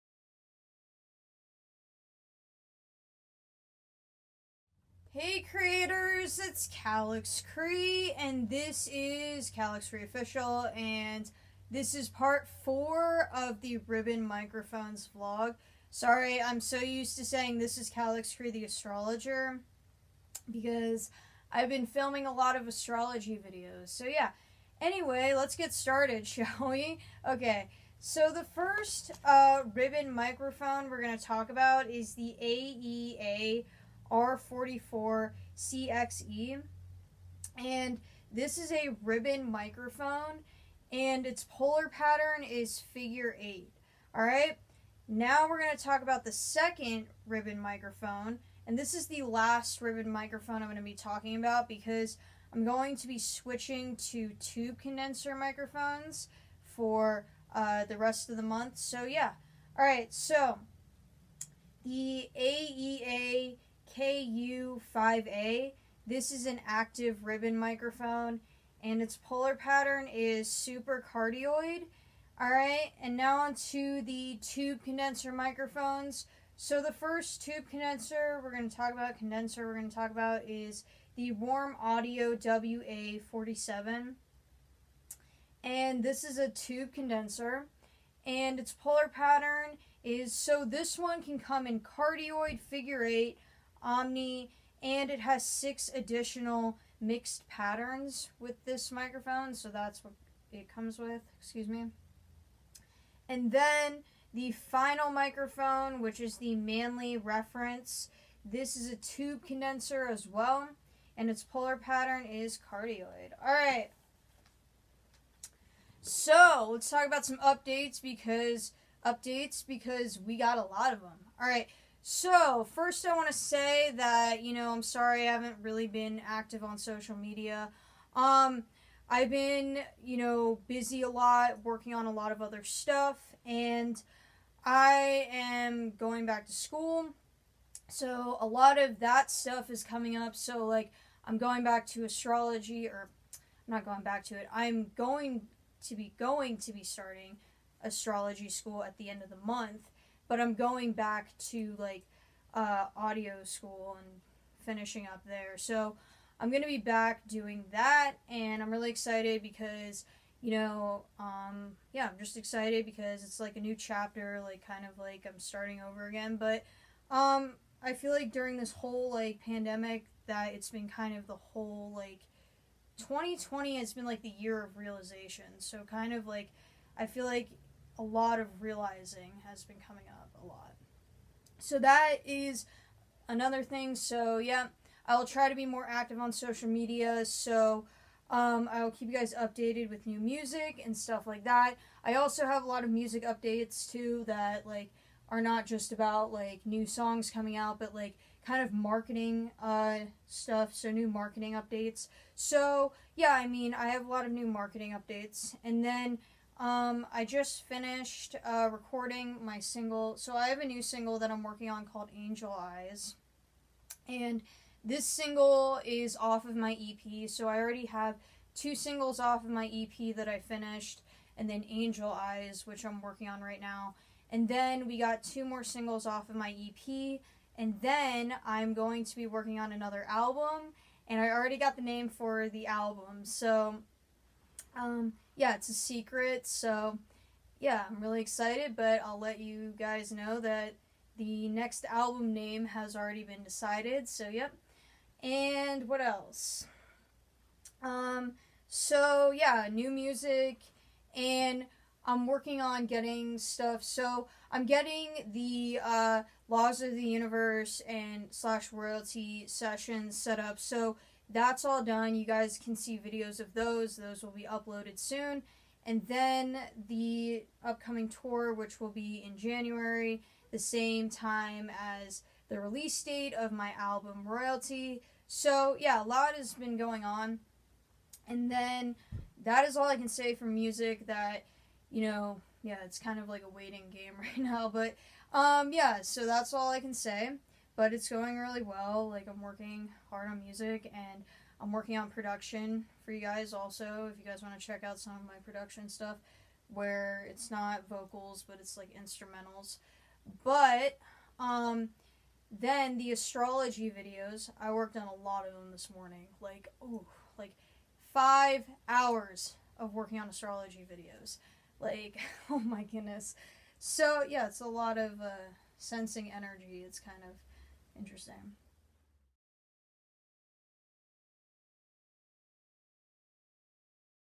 Ribbon Microphones/Tube Condenser Microphones Vlog
In this vlog, I’m wrapping up talking about Ribbon Microphones and starting the next playlist on Tube Condensers.

Checkout my music and the rest of my channel.